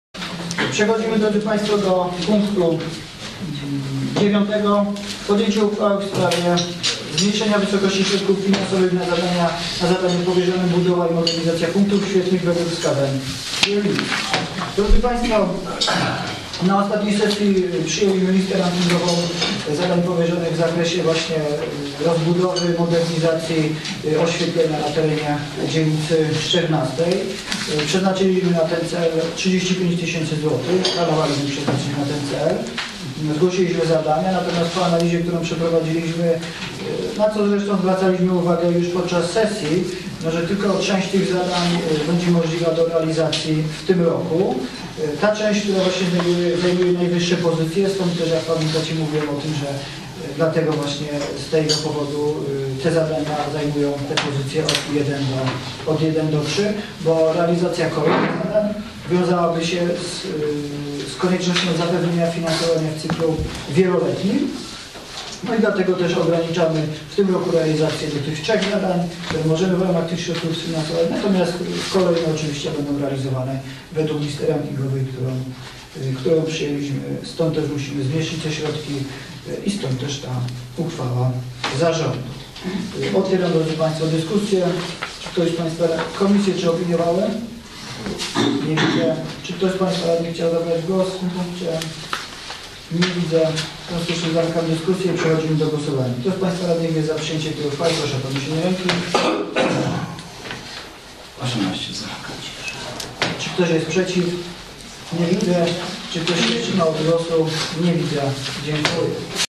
RD_XIV_14-03-2013_9 Zadanie powierzone - Budowa i modernizacja punktów świetlnych
Obrady XXVIII Sesji Rady Dzielnicy XIV Czyżyny z dn. 14 marca 2013. Nagrane przez Fundację Stańczyka w ramach projektu "Monitoring Rady Miasta i Dzielnic Krakowa", finansowanego ze środków "Demokracja w Działaniu" Fundacji im. Stefana Batorego.